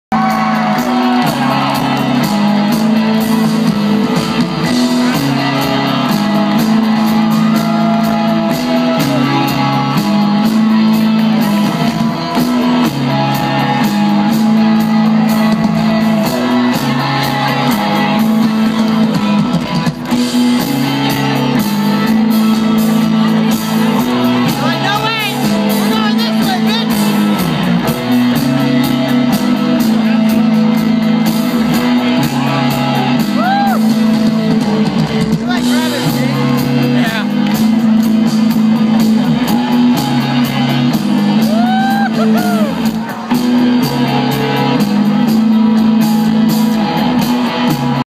Neon Trees Crowd Surfing
Neon Trees lead singer crowd surfs at Music Midtown 2012 in Atlanta, Ga